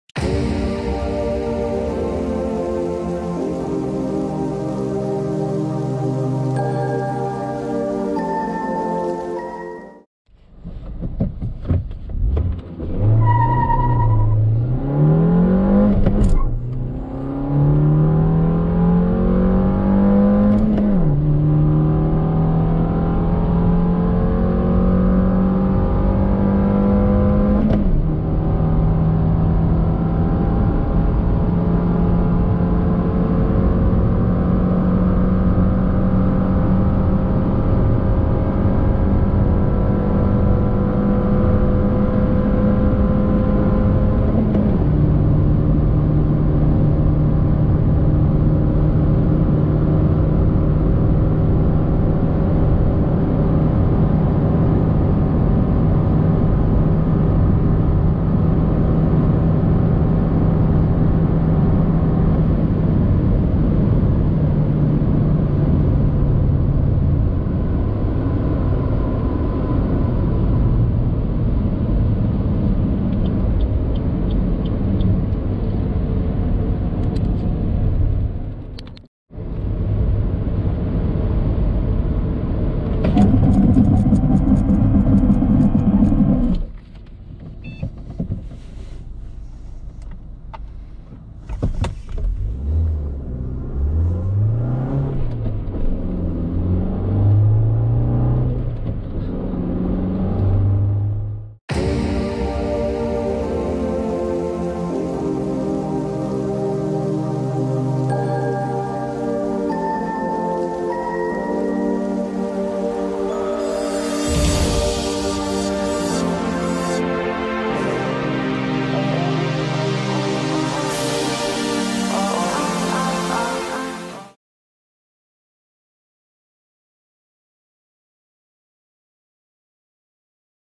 Acceleration, Brake & Top Speed Test: 2022 Toyota Rumion 1.5L
Racelogic PerformanceBox results for a MY22 Toyota Rumion 1.5 TX Manual

Manufacturer claims:
1462cc in-line 4-cylinder petrol
77kW @ 6000rpm
138Nm @ 4400rpm
5-speed Manual / FWD
0-100km/h: no claim
Top speed / v-max: 175km/h

Performance data / statistics should be in the video. Let us know which other cars we should test in the comments below and be sure to check out these links: